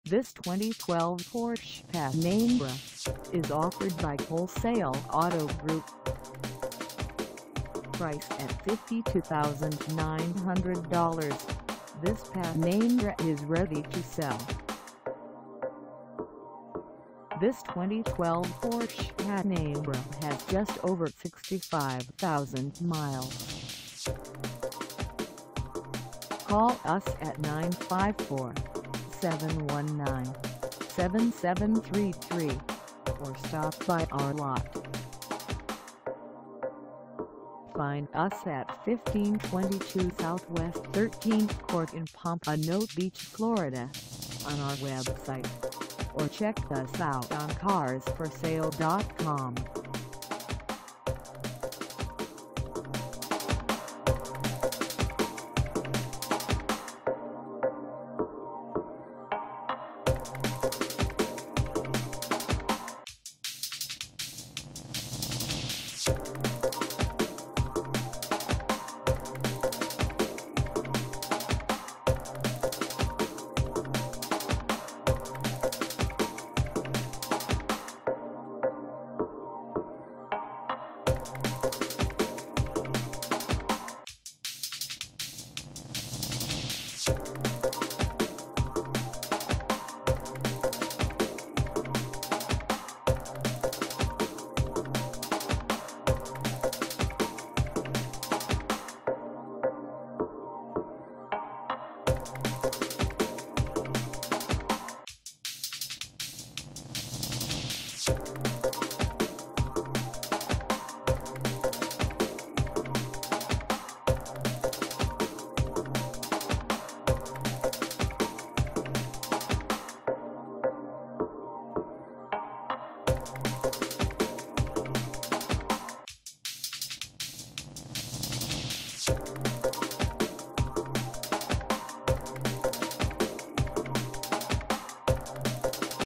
2012 Porsche Panamera for sale in Pompano Beach, FL 33069 at
This 2012 Porsche Panamera is for sale in Pompano Beach, FL 33069 at Wholesale Auto Group.
Engine: V6 3.0L V6 Supercharged
Transmission: Automatic 8-Speed
Color: Yacht Blue Metalic
Mileage: 65000
Contact Wholesale Auto Group 1522 SW 13th Court  Pompano Beach, FL 33069 to test drive this 2012 Porsche Panamera  today.